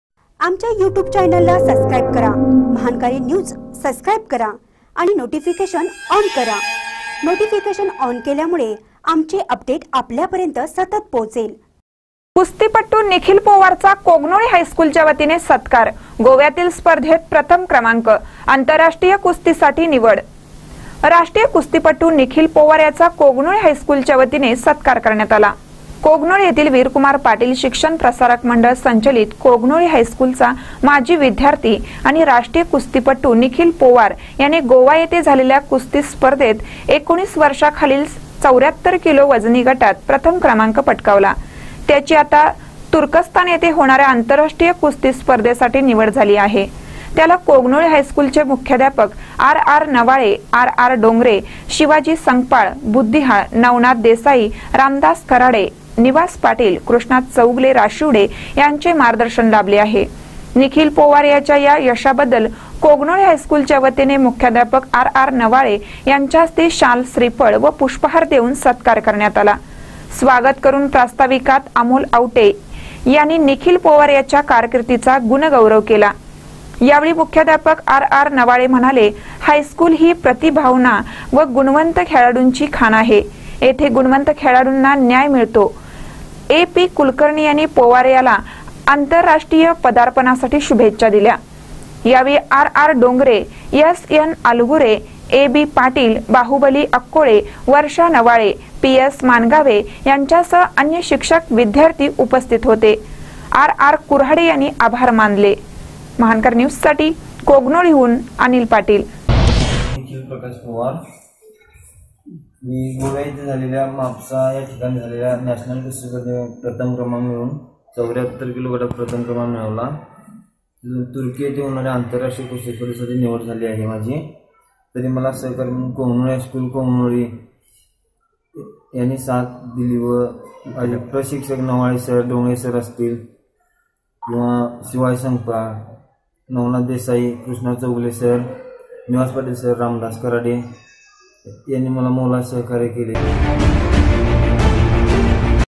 कुस्तीपटू निखील पोवारचा कोगनोळी हायस्कूलच्यावतीने सत्कार
कुस्तीपटू निखील पोवारचा कोगनोळी हायस्कूलच्यावतीने सत्कार : गोव्यातील स्पर्धेत प्रथम क्रमांक, आंतरराष्ट्रीय कुस्तीसाठी निवड